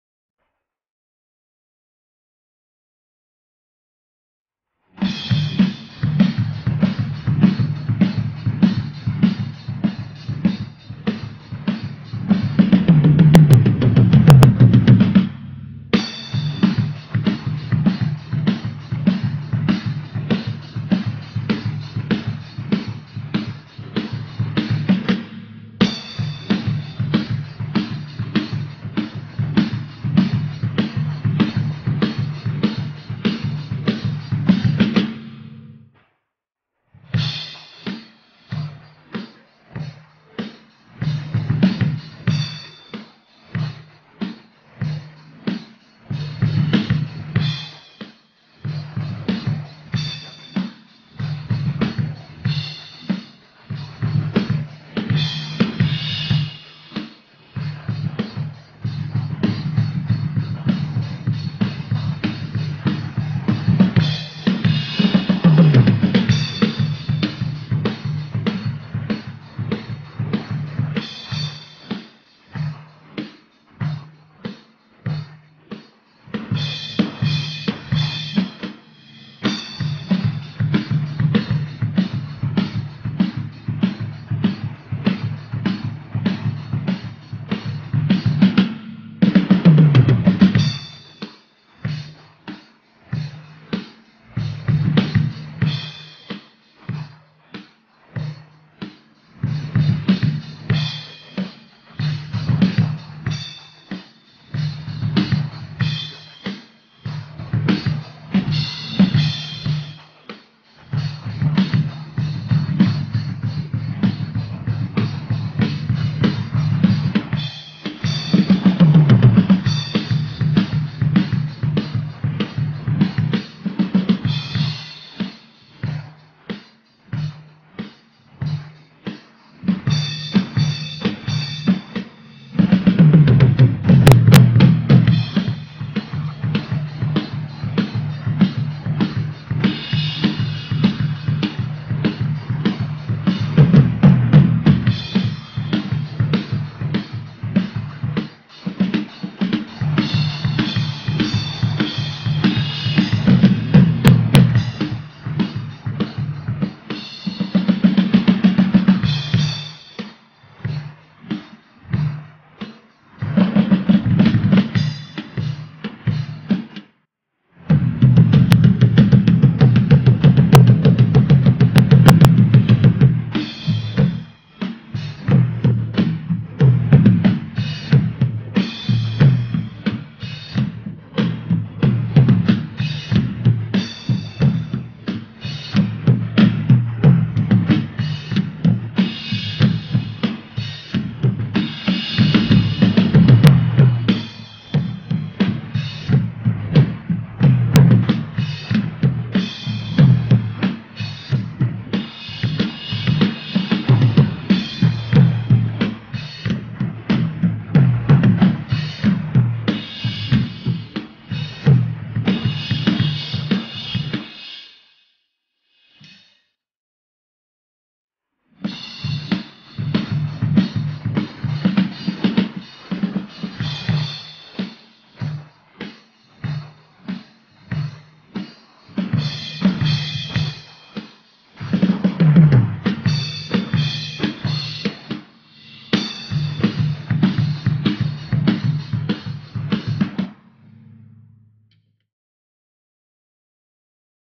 Niño 6 años experto tocando batería
Pedazo de canción se marca este niño.